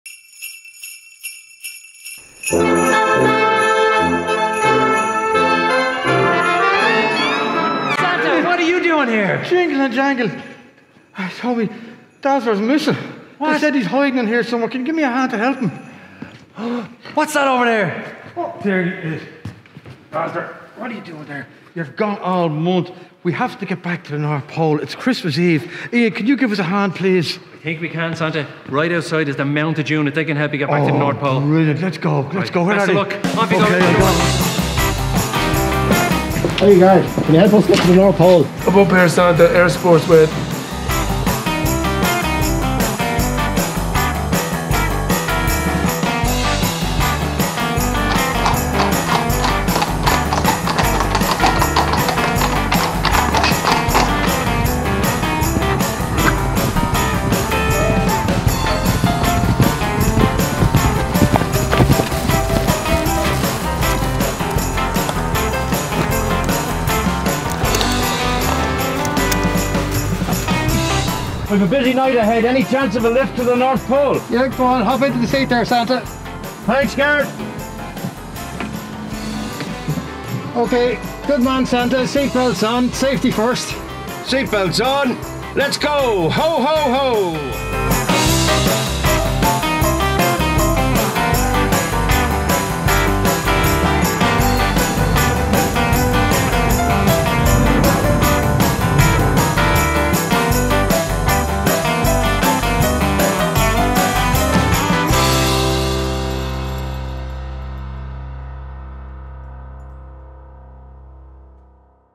We found Dazzler!!
As Santa sets off to the North Pole, he would like to thank everyone who has helped in the search and to remind children to go to bed early tonight. Wishing everyone a happy and safe Christmas and new year.

Agus Santa ag tabhairt aghaidh ar an Mol Thuaidh,ba mhaith leis buíochas a ghabháil le gach duine a chabraigh leis an gcuardach agus a mheabhrú do pháistí dul a chodladh go luath anocht.Ag guí Nollaig shona agus sábháilte ar gach duine agus athbhliain faoi mhaise